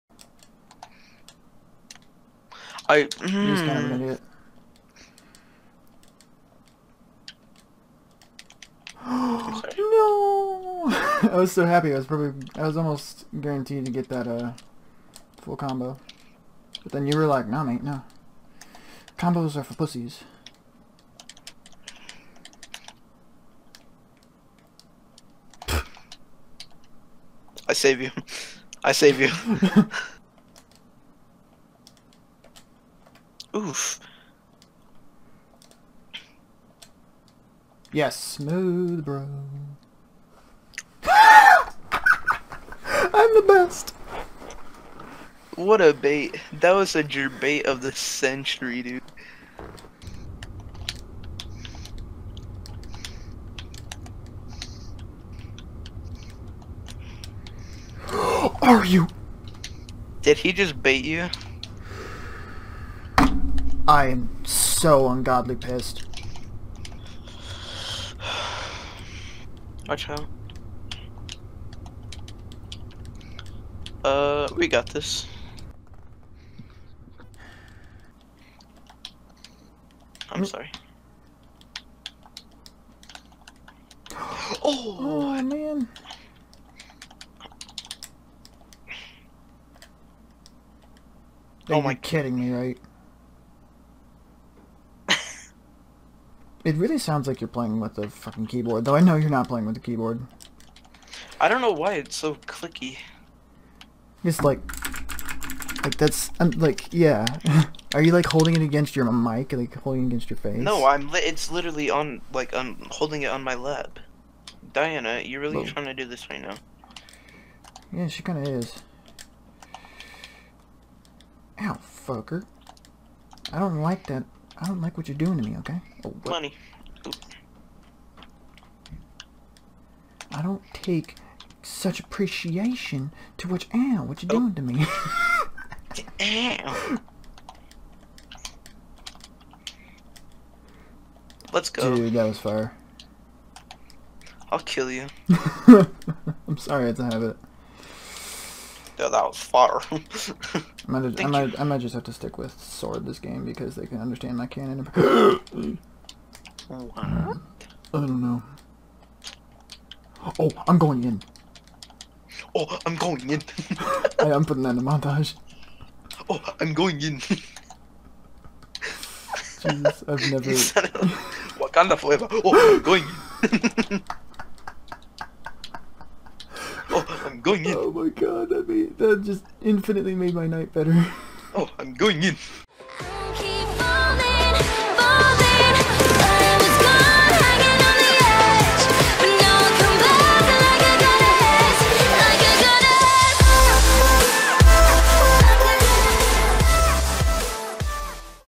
Ranked 2's w/ Bacca PT. 2
Another few matches of Bacca and I in ranked 2's. Its fun and very angering.